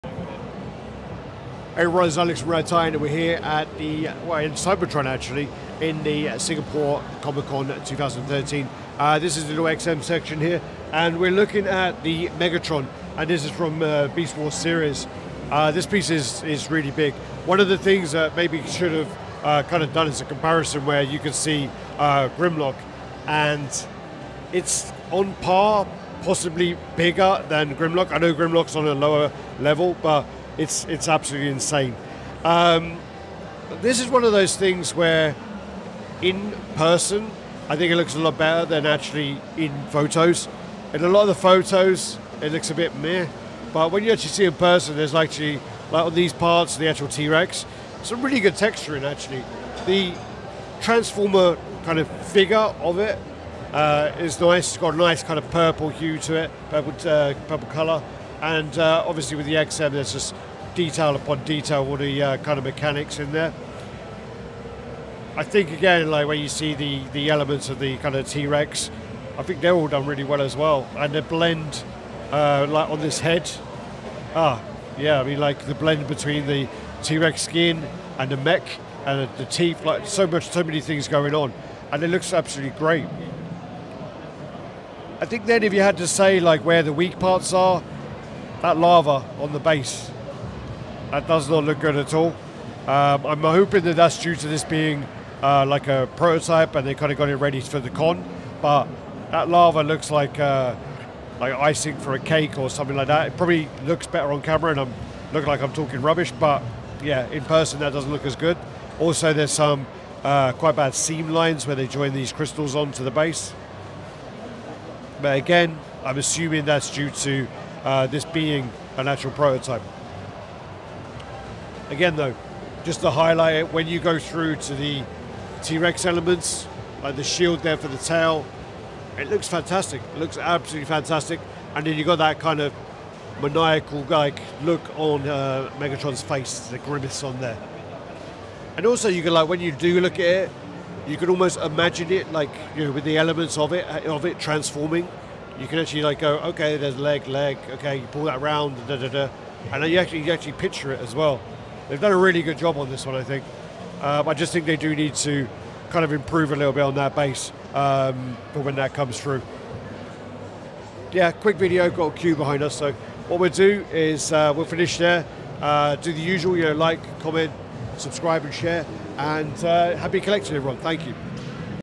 Exclusive First Look: XM Studios Transformers Beast Wars Megatron! 🤖🔥
"Exclusive First Look: XM Studios Transformers Beast Wars Megatron! 🤖🔥 Unveiling the Robotic Masterpiece!"

**Video Description:**
🔍 Get ready for a world-first glimpse into the future of collectibles! In this exclusive video, we're unveiling the XM Studios Transformers Beast Wars Megatron statue for the first time! Join us as we dive into the intricate details, jaw-dropping sculpting, and dynamic pose of this robotic masterpiece.

🤖 From the iconic Beast Wars series, Megatron takes center stage in this XM Studios creation, capturing the essence of the character in every inch. Witness the fusion of cutting-edge design, expert craftsmanship, and a passion for Transformers that brings Megatron to life like never before.

🌌 Marvel at the exclusive features, explore the meticulous paintwork, and see the statue from every angle as we provide an in-depth first look at this collector's dream. Whether you're a Transformers fanatic or a casual admirer, this video is your ticket to a futuristic world of awe-inspiring collectibles.

🌟 Don't miss out on being among the first to witness this groundbreaking statue.

XM Studios, Transformers Beast Wars, Megatron Statue, Exclusive First Look, Collectibles Unveiled, Robot Masterpiece, Transformers Fanatics, Collector Insights.